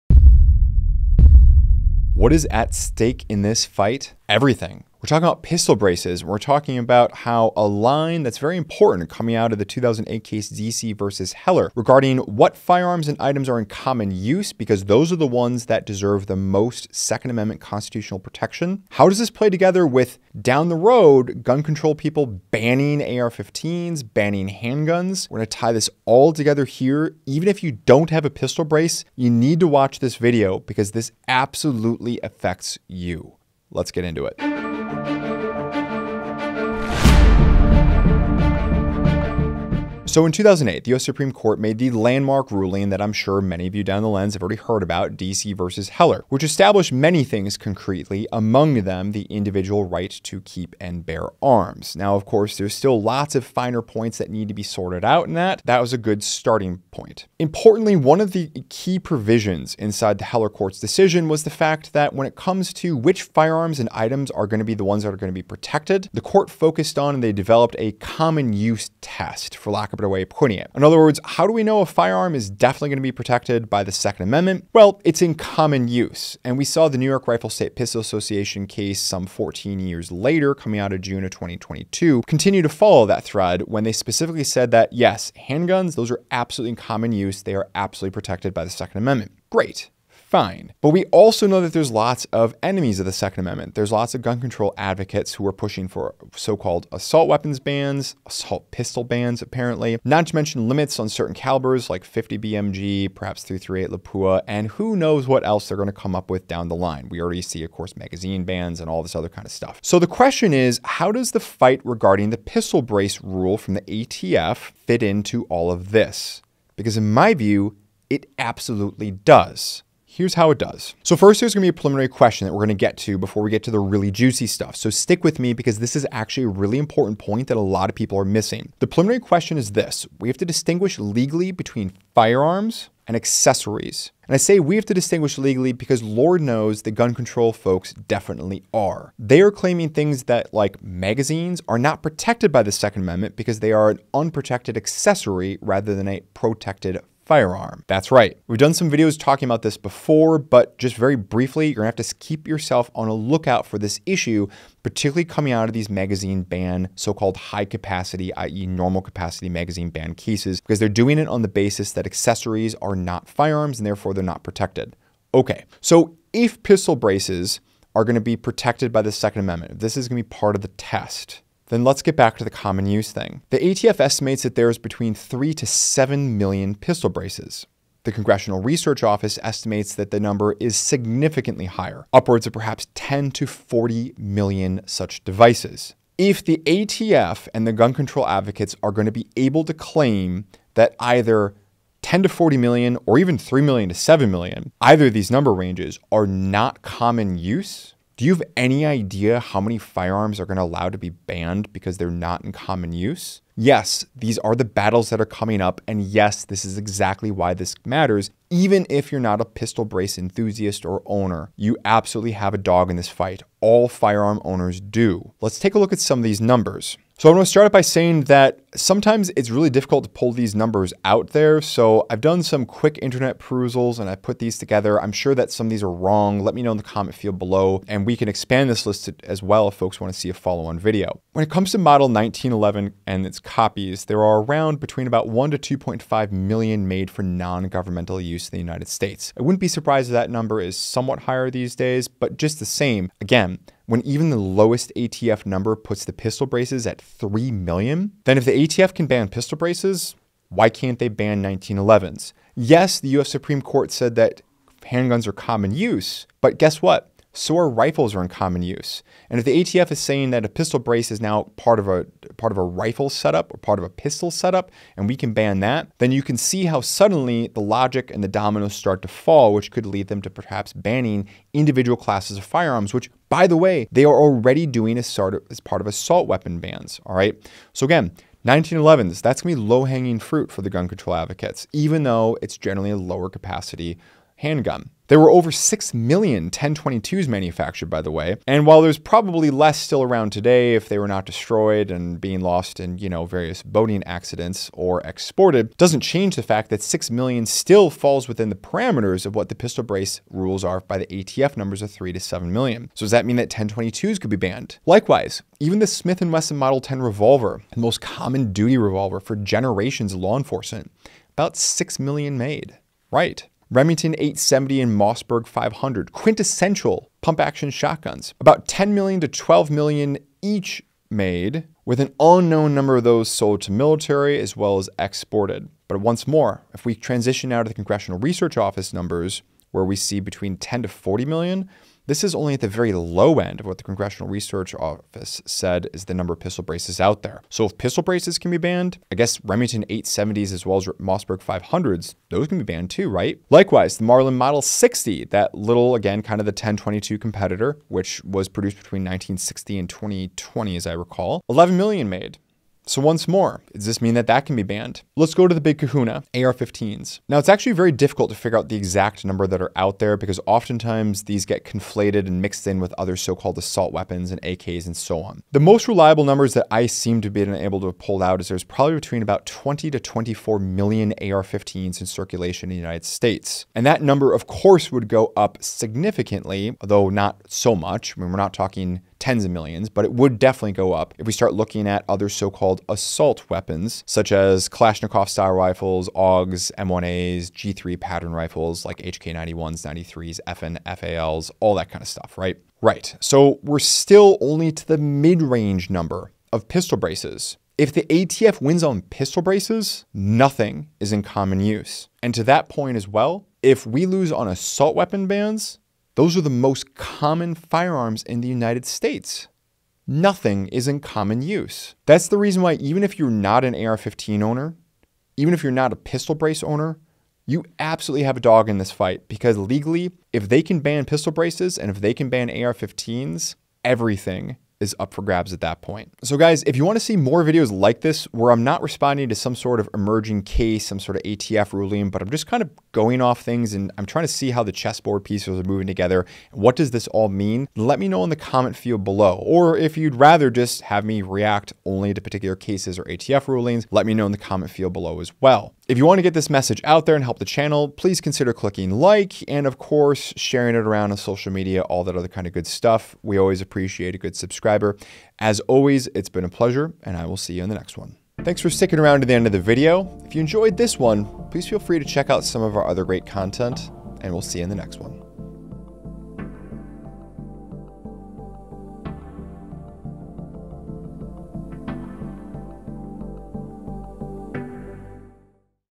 This ATF Pistol Brace Ruling Puts EVERY GUN At RISK Of Being Banned?!
Ex-prosecutor Tom Grieve explains what is up for grabs, beyond the obvious pistol braces, in all of these pistol brace law suits.  Specifically, what does an important phrase from DC v. Heller (2008) have EVERYTHING to do with AR15s and other popular weapons.

Learn about firearms v. devices and why "common use" are two of the most important words in the legal battles over the 2nd amendment.